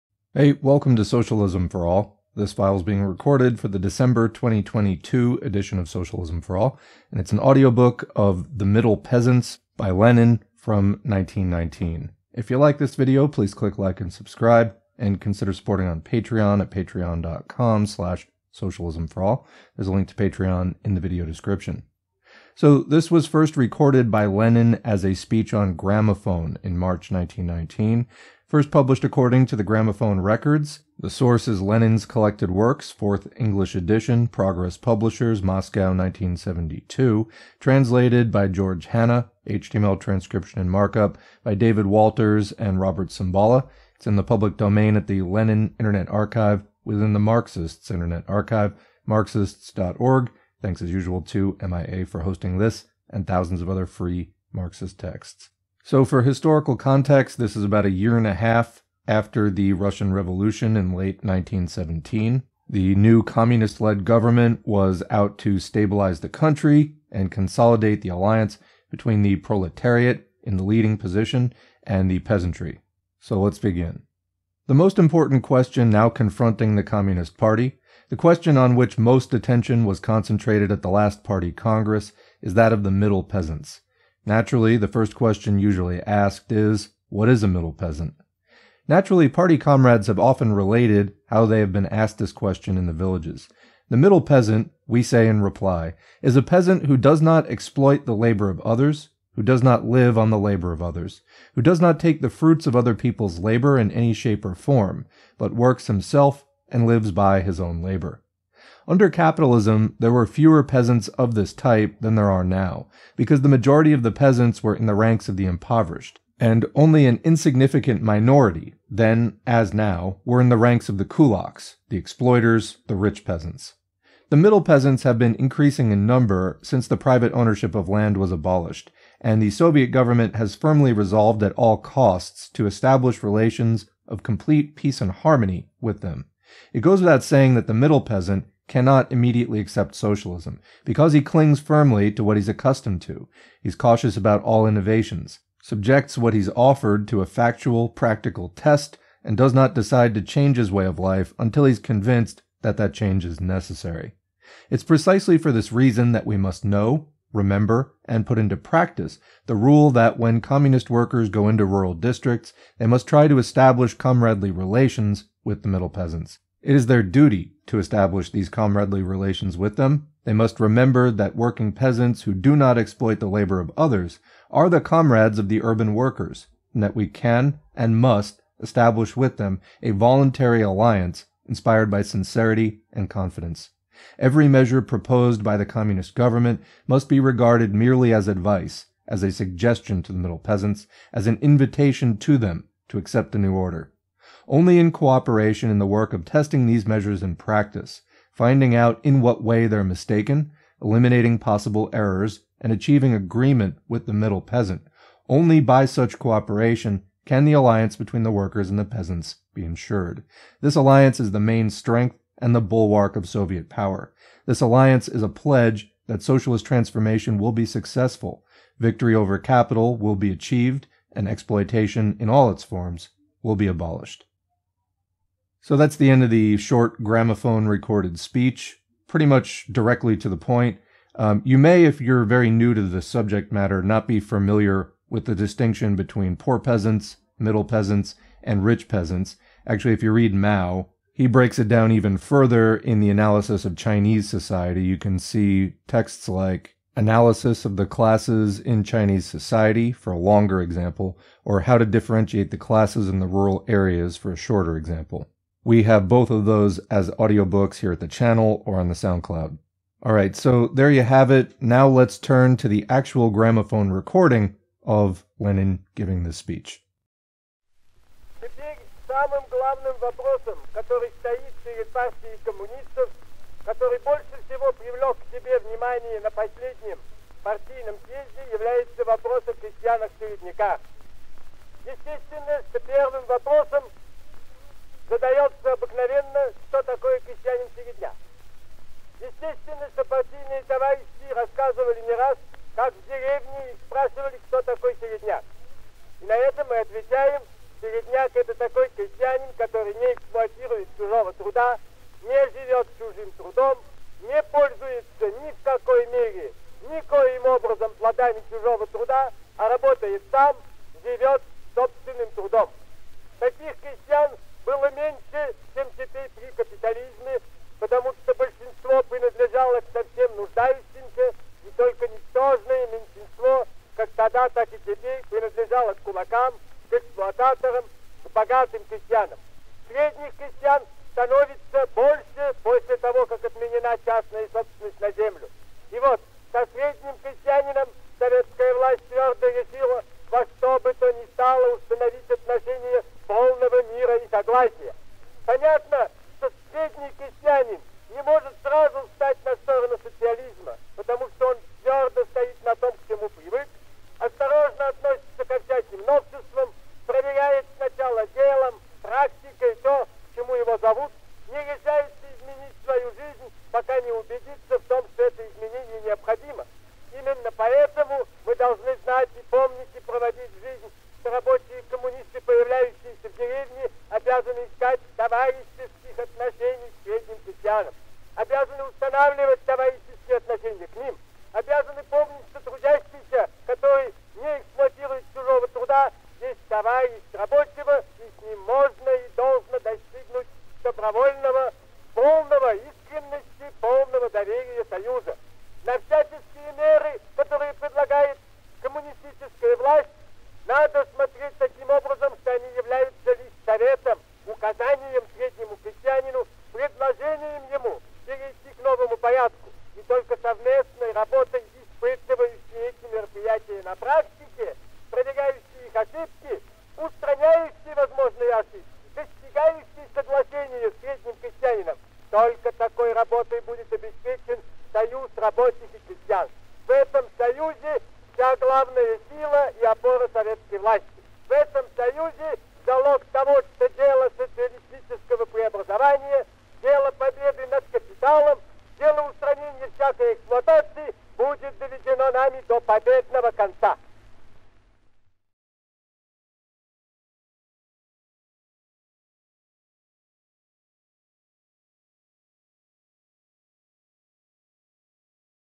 "The Middle Peasants" (1919) by Lenin. Marxist/Socialist Audiobook + Original Gramophone Recording.
Socialism for All does not run ads on its material.

Timestamps:
0:00 S4A Intro & Front Matter
1:14 Audiobook
3:45 S4A Comments
4:26 Lenin's Speech from Gramophone
7:25 Thanks to the Patrons